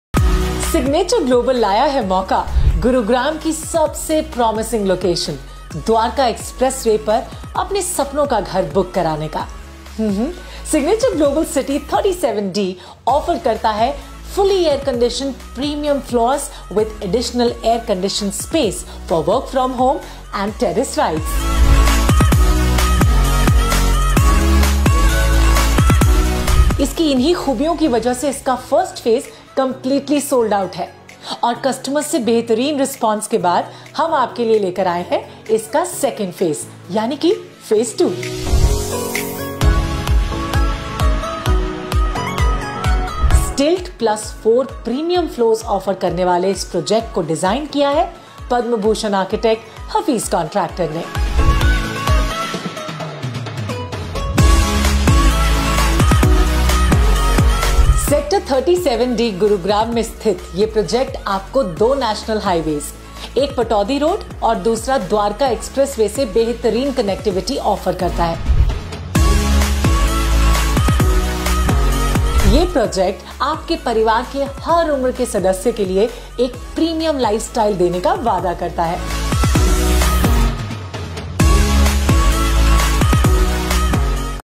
Luxurious 2 & 3 BHK @ ₹ 82 L* - Signature Global 37D, Gurugram
Signature Global Presenting 2/3 BHK In Sector 37d Gurgaon. Luxury Floors In Gurgaon with Lift + Dedicated Car Parking + Terrace Garden. Best Location. Call Us @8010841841 Book Appointment. Get The Best Rate From Us.

👉 25+ Luxury Amenities
👉 Well established network of Schools, Hospitals, Shopping Malls, Retail Markets and Five Star Hotels
👉 20 mins from IGI Airport via Dwarka Expressway.
👉 Proposed metro station in sec 37D.
👉 Air-conditioned Home Office With Waiting Lobby
👉️ Fully Air-conditioned Premium Floor